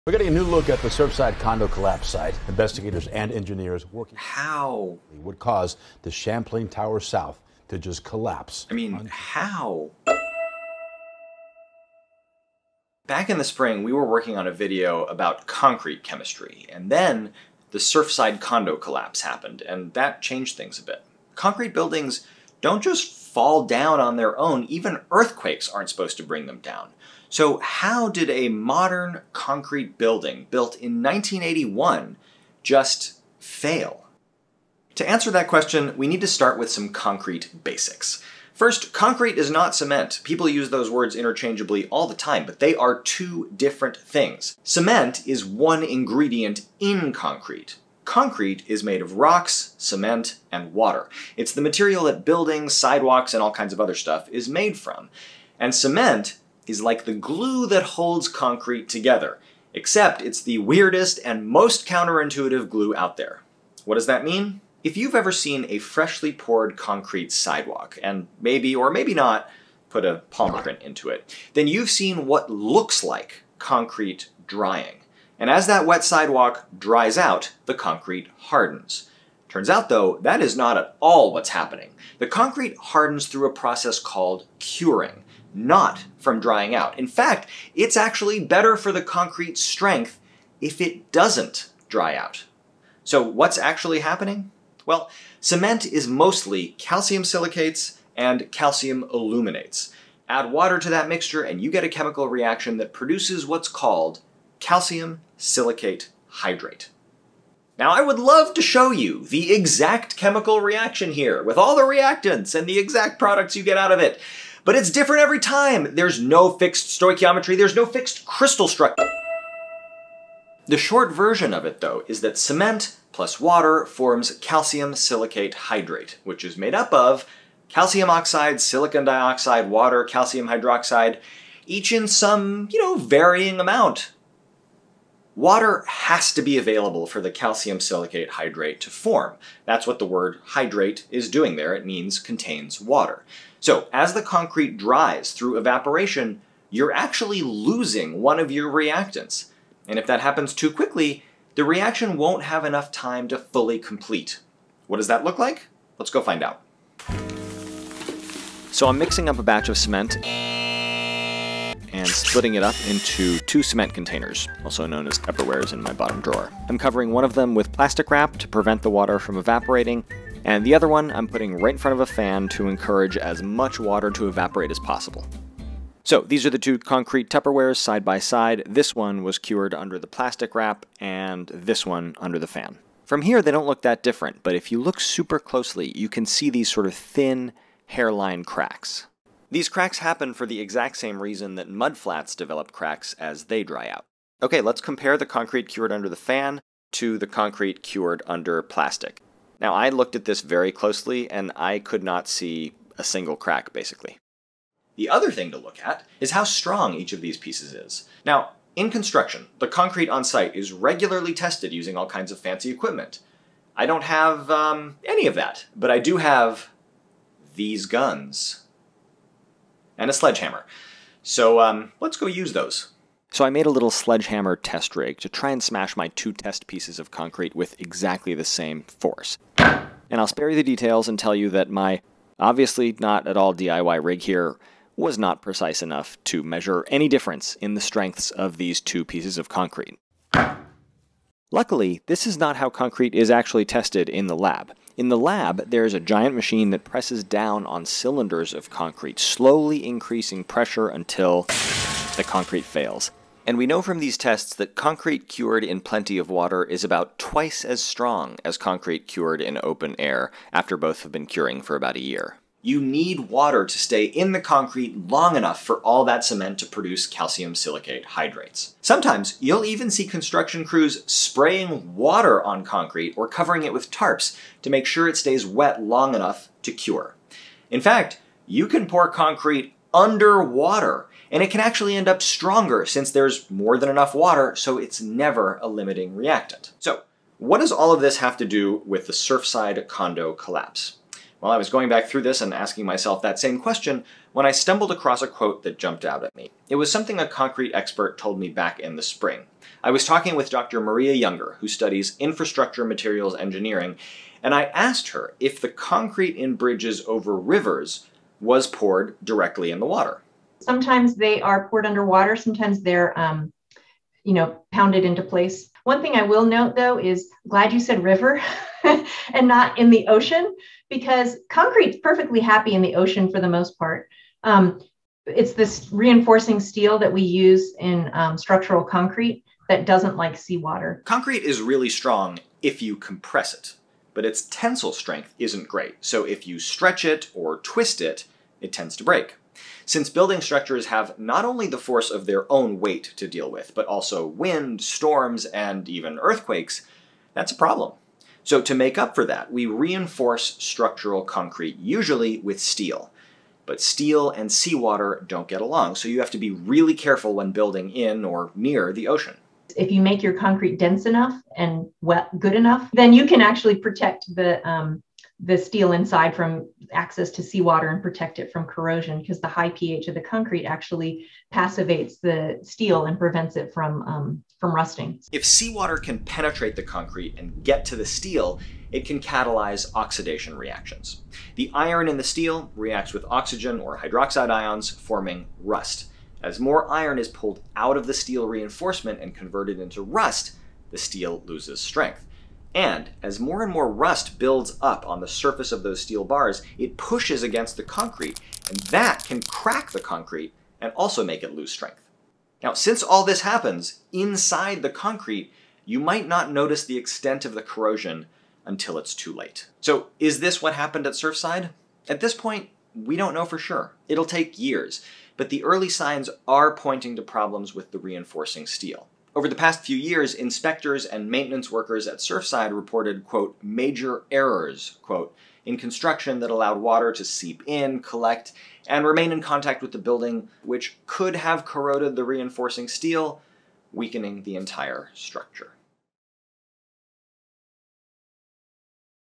What Happened in the Surfside Condo Collapse?
Even earthquakes aren’t supposed to bring down concrete buildings. So how did the Surfside Condominium—a modern building built in 1981—fail?

Credits:
Executive Producers:
George Zaidan
Hilary Hudson

Producers:
Laurence Vuckovic
Andrew Sobey
Darren Weaver
Elaine Seward

Host:
George Zaidan
Sam Jones, PhD

Writer:
Andrew Sobey

Scientific consultants:
Michelle Boucher, PhD
Maria Juenger, PhD
Brianne Raccor, PhD

Sources:
Champlain Towers structural field survey:
Quotes from the top half of pg 7:
"failed waterproofing is causing major structural damage"
"concrete slab is not sloped to drain... This is a major error in the development of the original contract documents prepared by [the architects]"

Ex-Maintenance Manager for Surfside Condo That Collapsed Recalls Saltwater Intrusion

‘The building was in OK shape.’ The upscale condo near Miami Beach still collapsed.